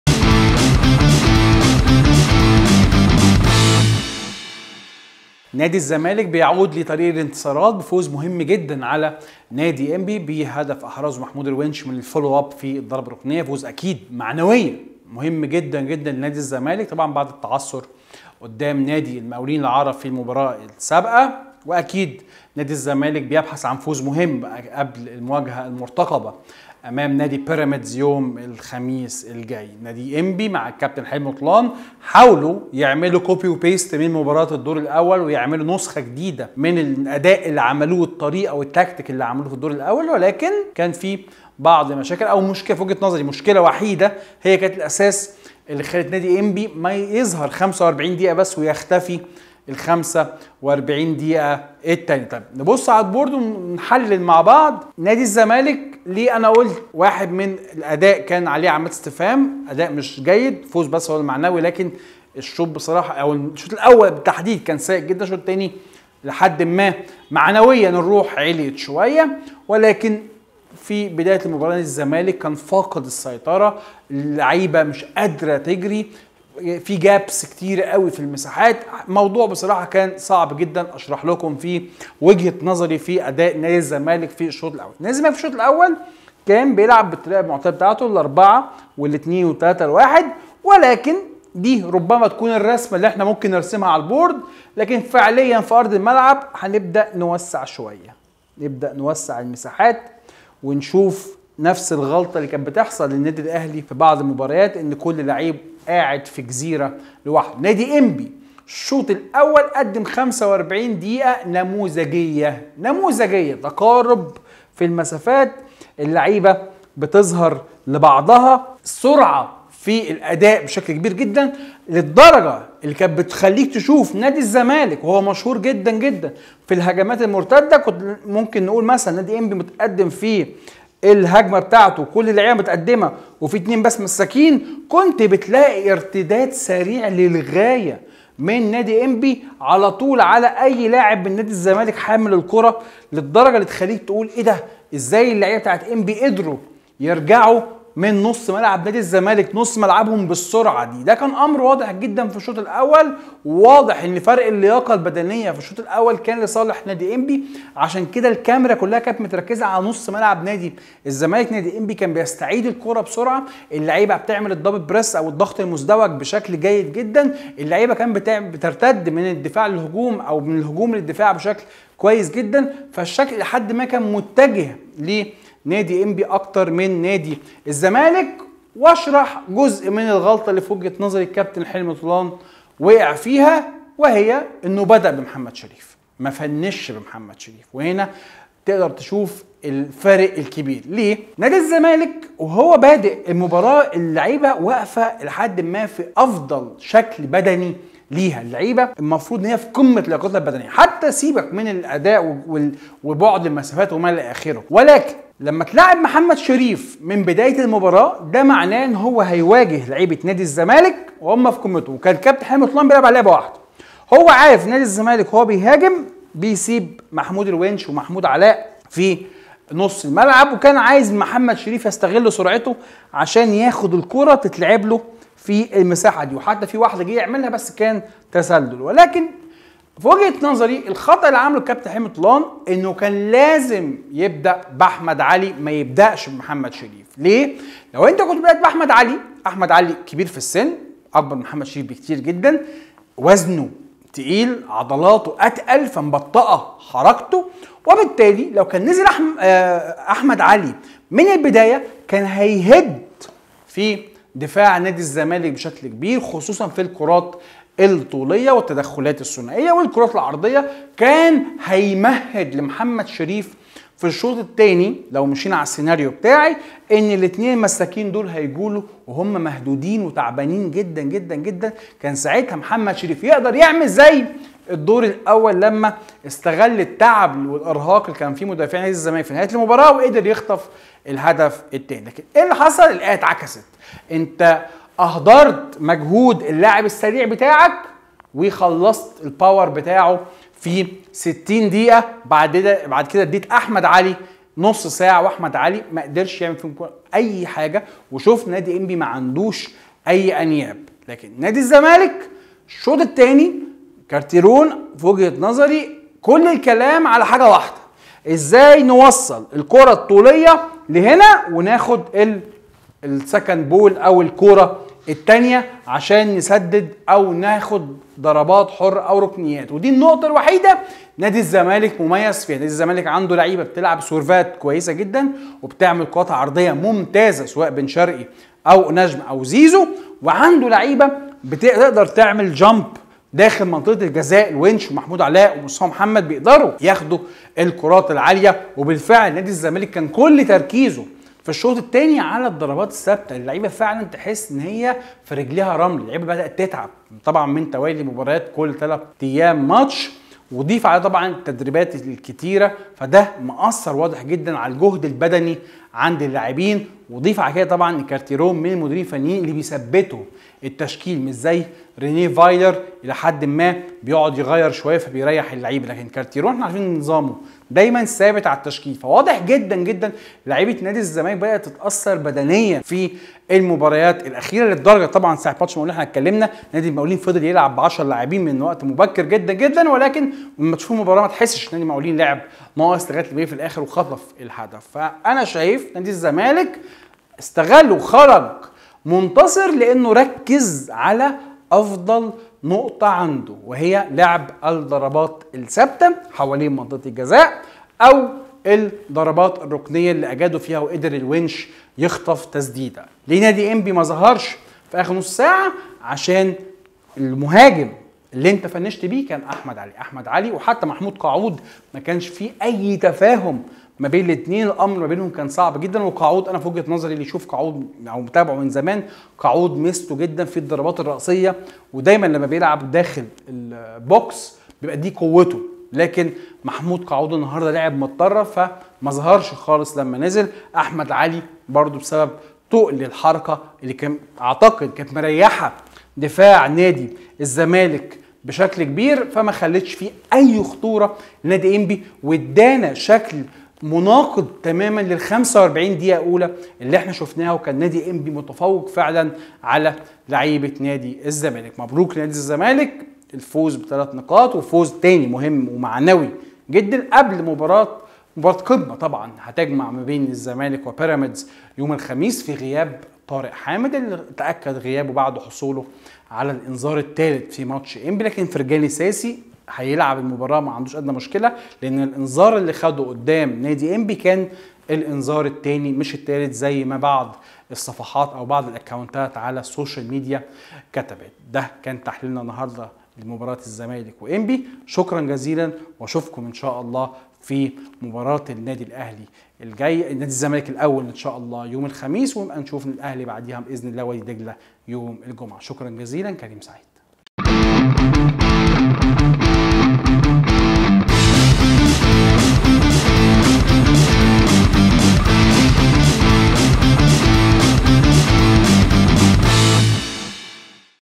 تريند_يلاكورة.. كريم سعيد يحلل: فوز معنوي فقط للزمالك على انبي.. وغلطة طولان في المباراة
كريم سعيد يحلل مباراة الزمالك وانبي والامور الفنية لكارتيرون وحلمي طولان